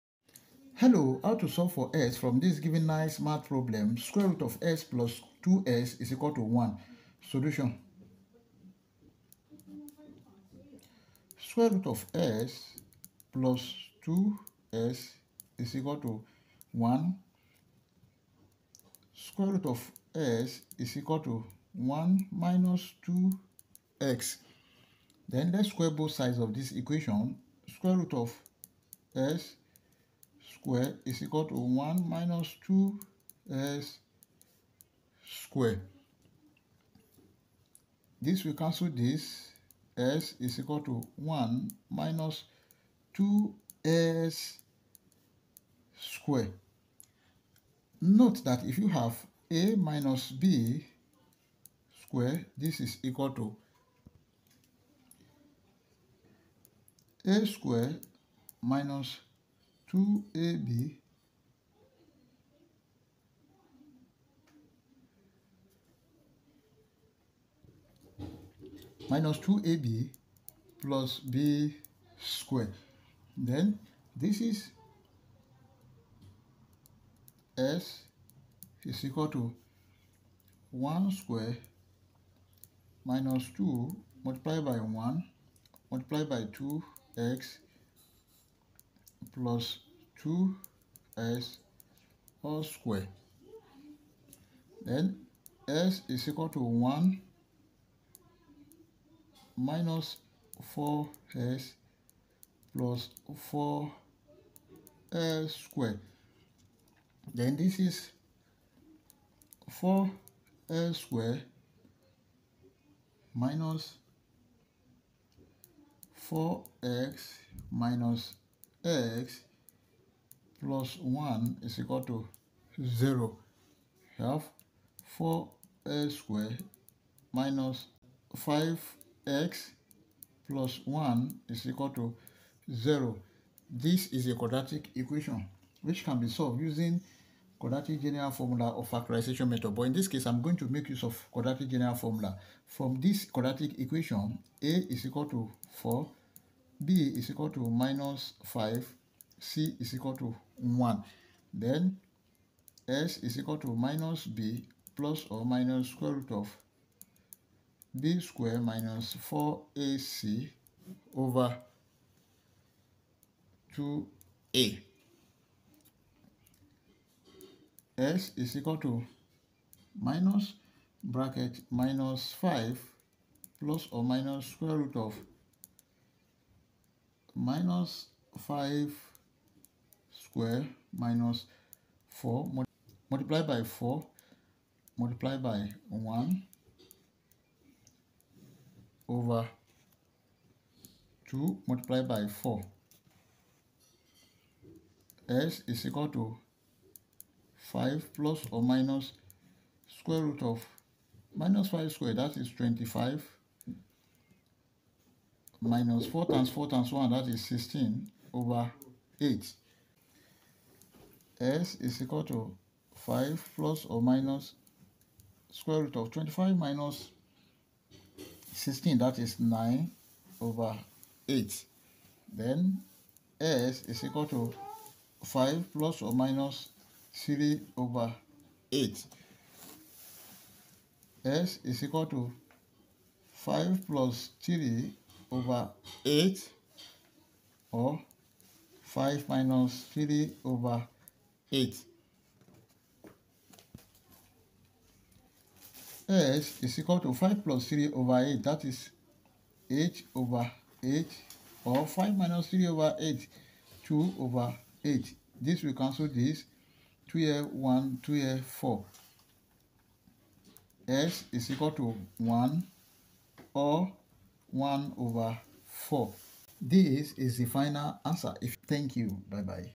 A Beautiful Math Problem √x + 2x = 1
This video will teach you how to solve the given nice Olympiad problem very fast.